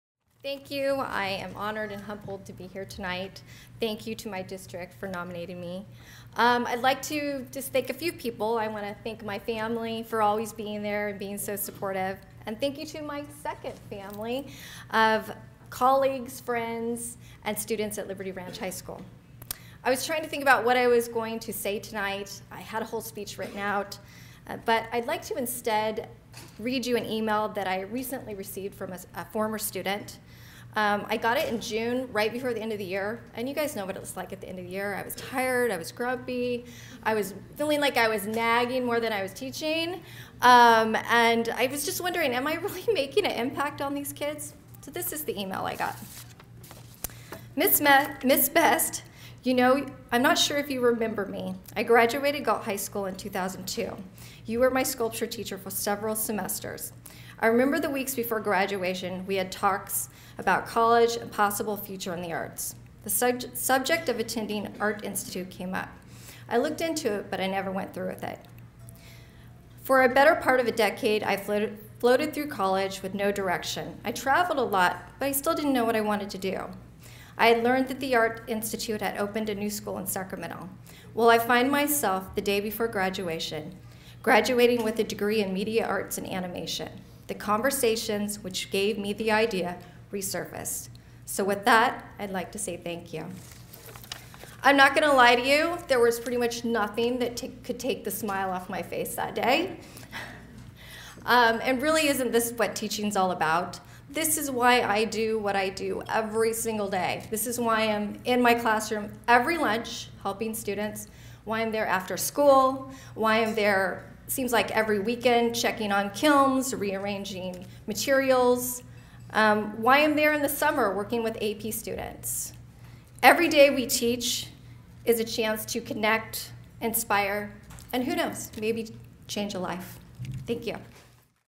Shanee Best: Sacramento County Teachers of the Year 2016 Awards Speech
The Sacramento County Teachers of the Year program recognizes excellence in teaching within the county's schools. Participating districts nominate individual teachers to receive this special recognition. This speech was delivered at the Teachers of the Year awards recognition dinner where two teachers were identified as the Sacramento County Teachers of the Year and became eligible to participate in the statewide competition.